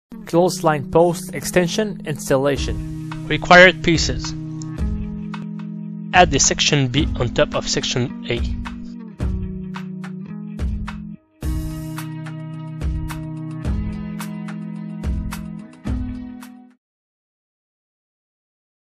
48in Clothesline post extra section • Item No. 10738
Central section for clothesline post, give the opportunity to extend the post of an extra 4 feet. Include 1 section and bolts.